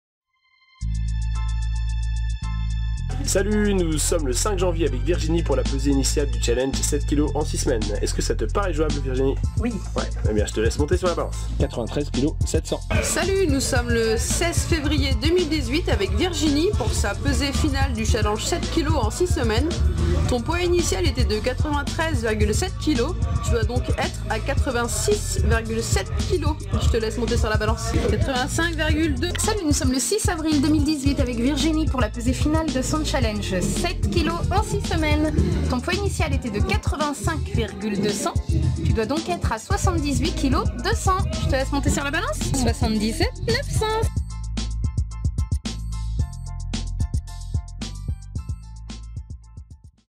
Transformation physique rapide Angers
Virginie a perdu 15,800 kgs en 12 semaines à la Training Academy